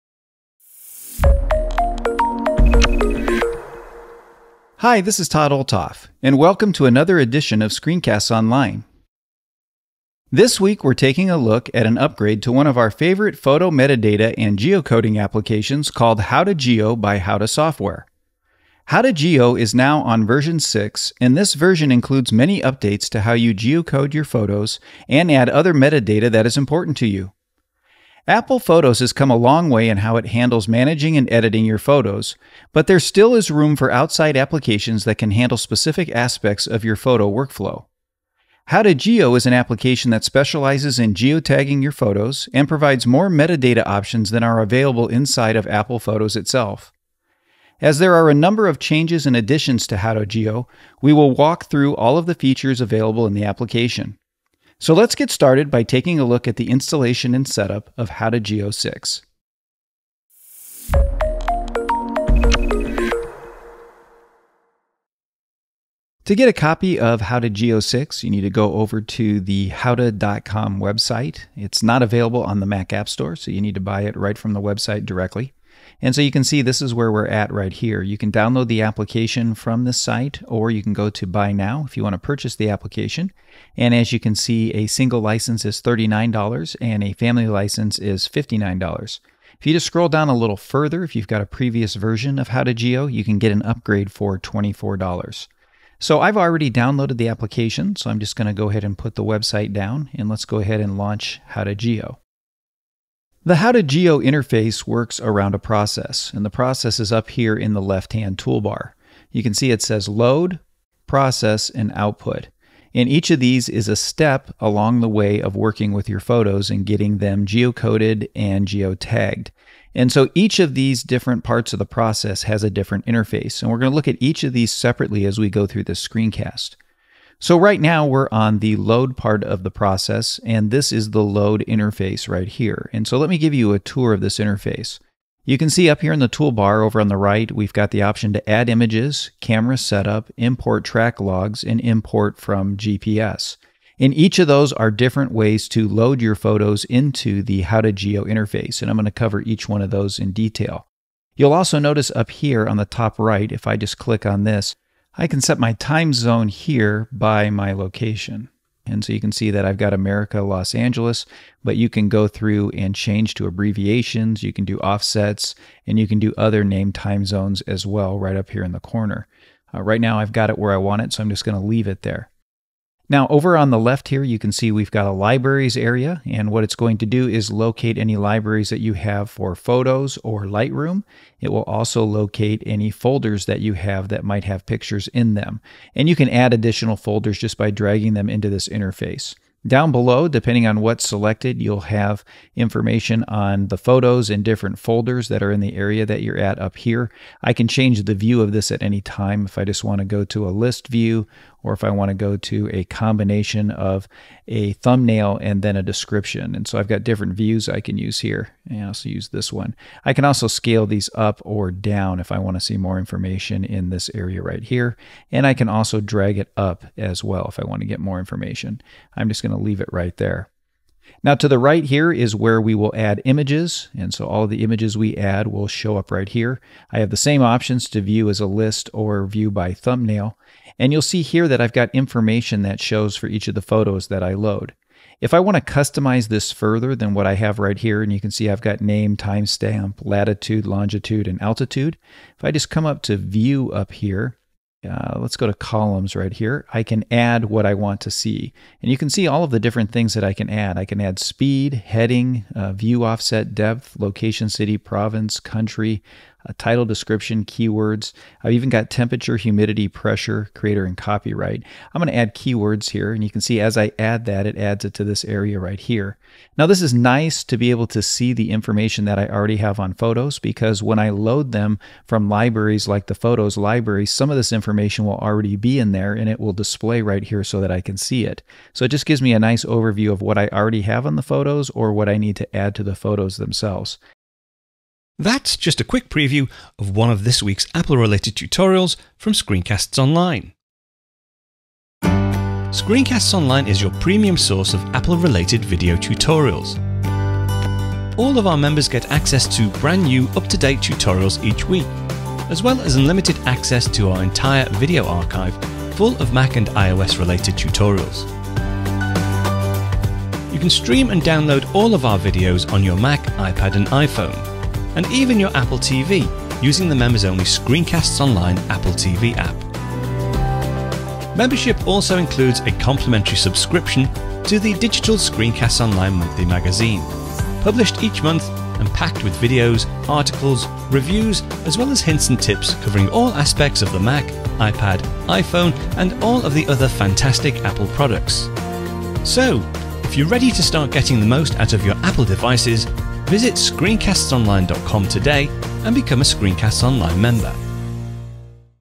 SCOM0933 - HoudahGeo 6 Update - Preview
This is a short preview of a full ScreenCastsOnline video tutorial. for a free 7-day membership. - Modern-day photo management usually involves choosing an app and then managing all of the metadata for your photos within it. Using that metadata across applications is sometimes problematic because that information lives inside the original application and cannot always be shared easily.

HoudahGeo 6 is a program that streamlines the process of adding geocode and other metadata to your files. It can communicate with photo apps like Photos and Lightroom to make sure any changes made to your files show up inside those applications as well.

After demonstrating how to get your photos in the application, Todd Olthoff covers all of the geocoding and metadata options, how to add that data to your files, and then shows you how to send this information back to your photo library.